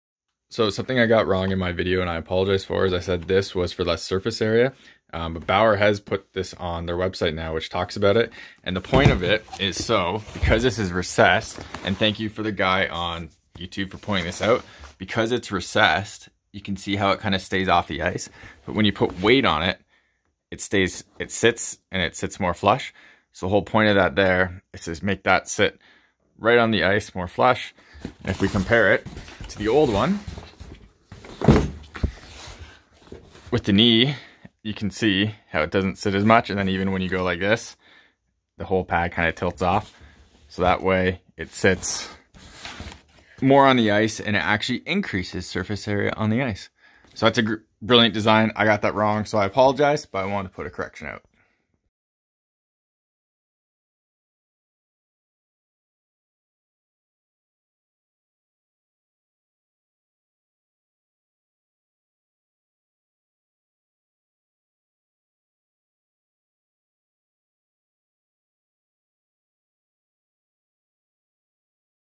Bauer Supreme Ultrasonic Stabilislide example & review correction
I made a mistake in my review video of the Bauer Supreme Ultrasonic pads about the calf section and "less surface area" with the angled calf design...

Turns out I was totally wrong and it actually ADDS surface area by flattening itself out so it is on the ground. Here is an example of that.

I apologize for the mistake and it shouldn't have happened.

Equipment Used:
Pads: Bauer Supreme Ultrasonic, Bauer Vapor 2X Pro
Camera: Samsung Galaxy S10
Microphone: Robe Videomic GO